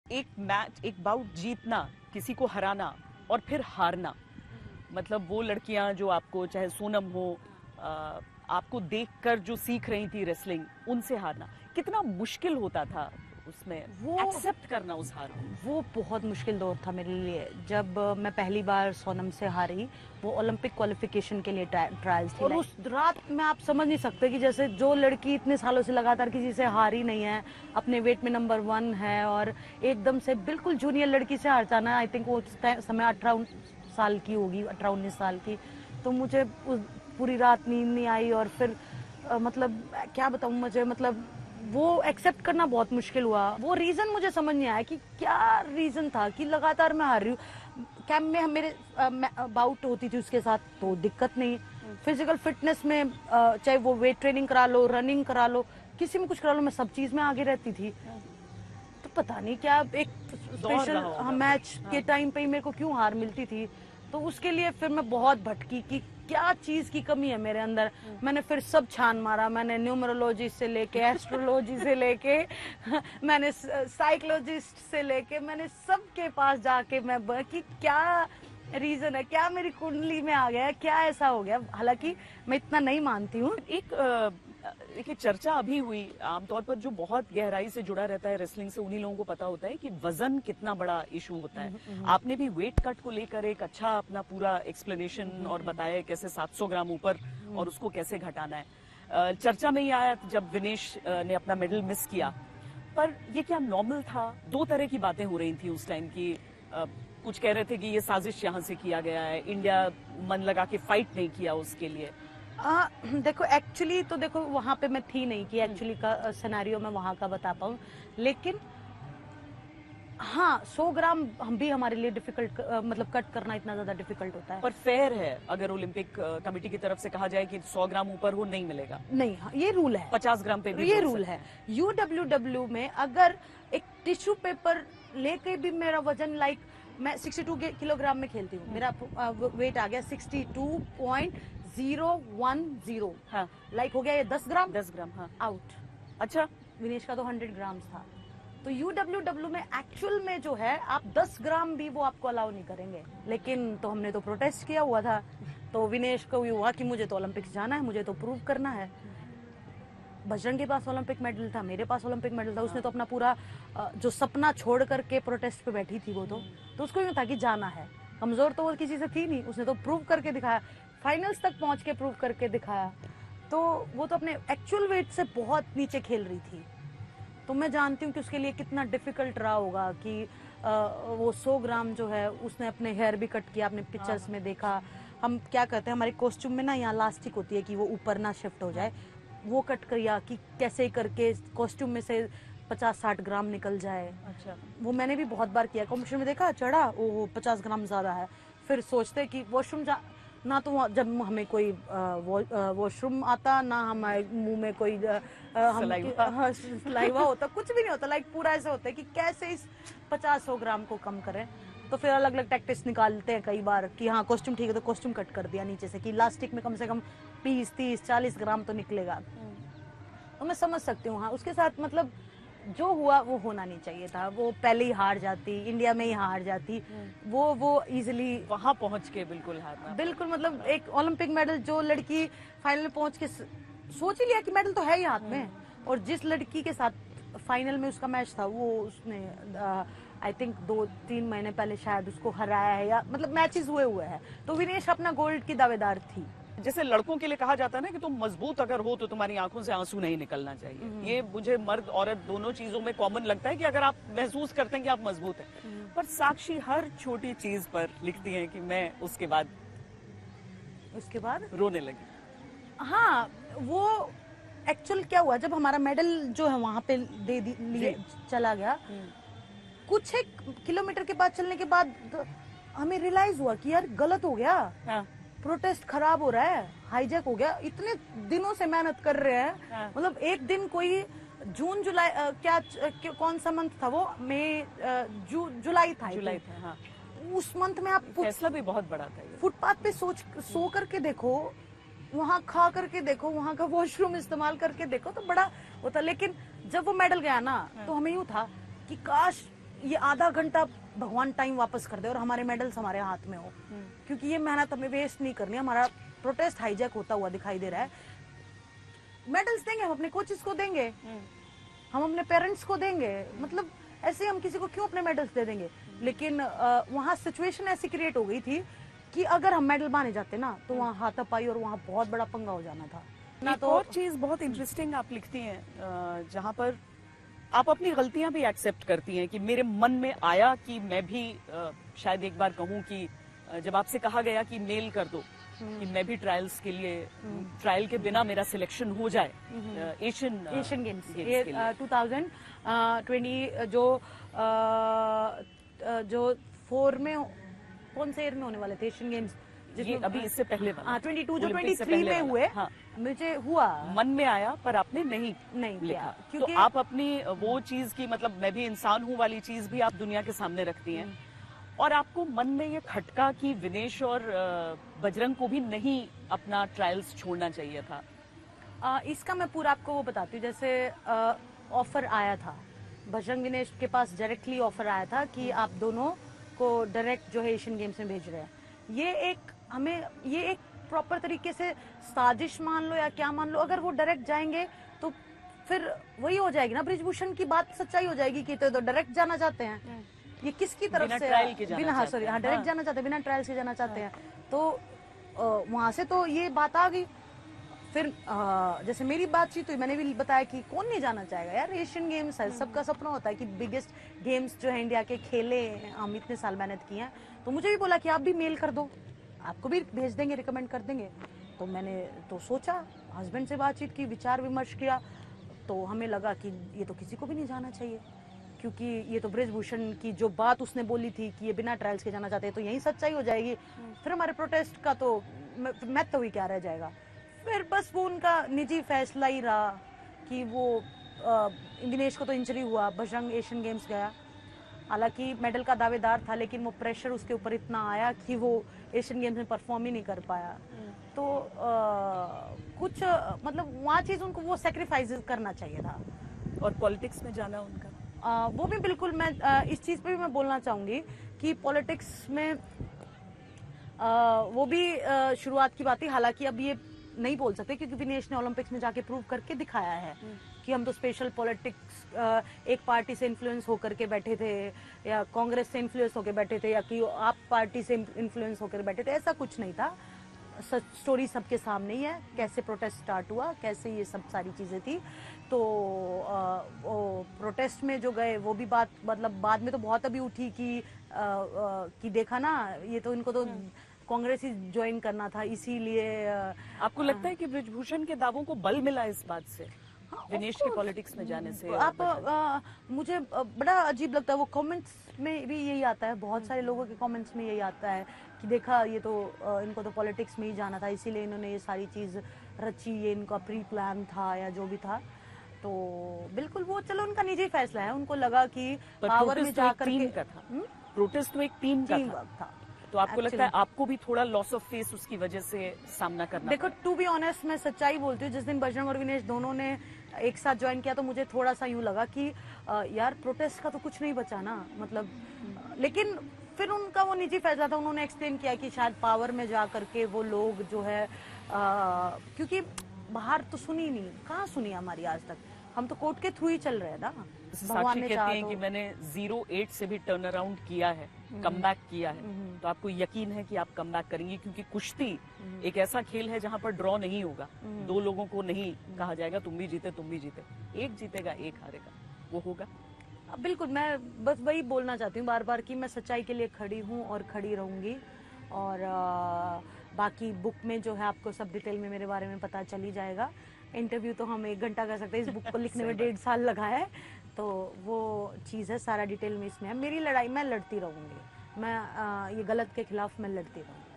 Sakshi Malik Exclusive Interview: Vinesh, Bajrang के राजनीति में जाने पर क्या बोलीं Sakshi Malik?
भारत के लिए ओलंपिक मेडल जीत चुकीं महिला पहलवान साक्षी मलिक ने अपनी आत्मकथा में हर मुद्दे पर लिखा साथ ही उन्होंने विनेश के राजनीति में जाने पर भी खुलकर अपनी बात रखी.